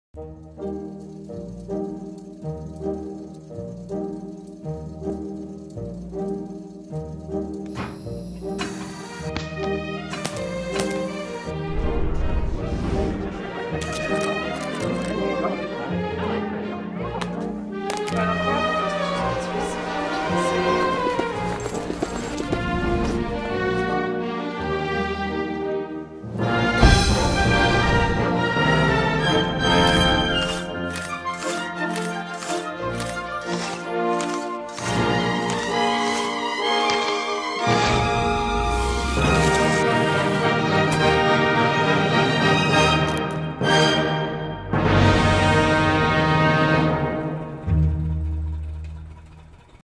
This is what Otterbox cases go through - the ultimate drop test
What do 24+ tests and 238+ hours of testing mean to you? They mean your OtterBox case is ready to dive into your day without a second thought — just like you are. OtterBox Certified Drop+ Protection means drops, bumps and fumbles are just the beginning. It means your device is protected from the barrage of wear & tear you subject it to everyday. So go ahead — do what you do and leave the protection to us.